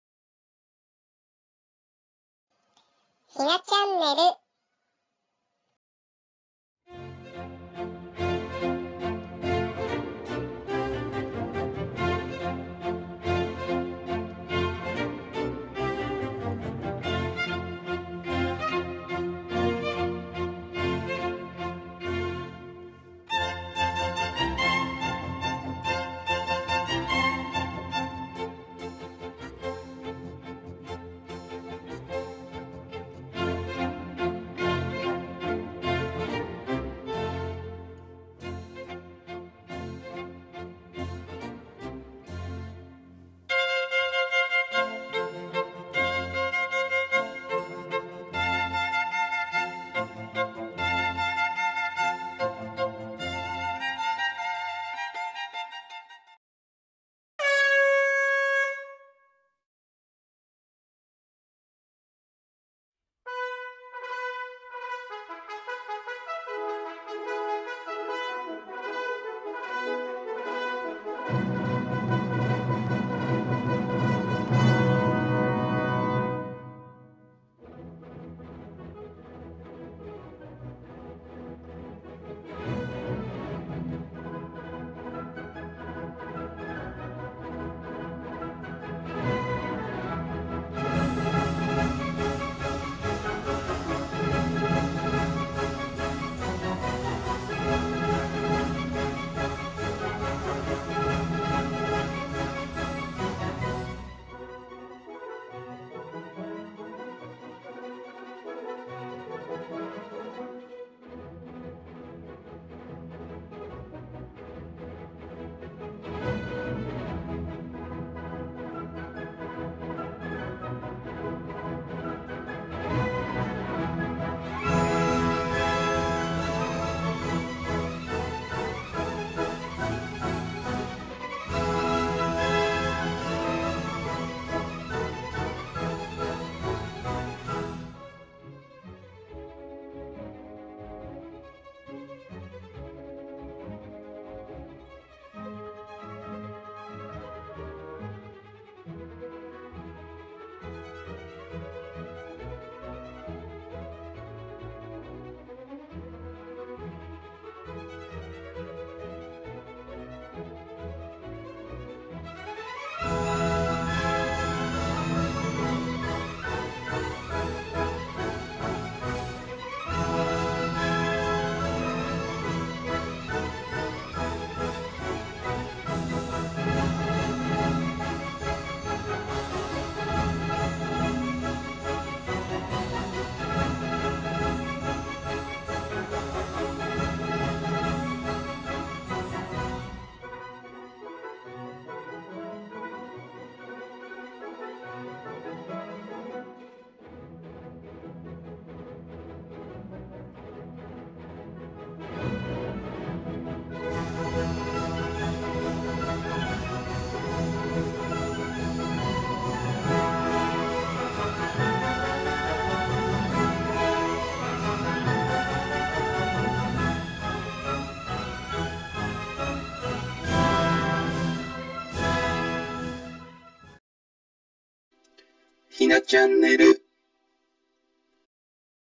🐹寝起きのむー君♂️にひなちゃん♀️ハウスでお留守番させたらテンションMAXになった(ジャンガリアンハムスター)
ジャンガリアンハムスターの男の子を女の子のお部屋でお留守番させた時の動画です。
はしゃぐ姿が可愛らしいです😊

寝る前の癒しのひとときになれば、より幸いです。

ひなちゃんねる

Hi!
This video is about my cute djungarian hamsters.
Thank you for coming today!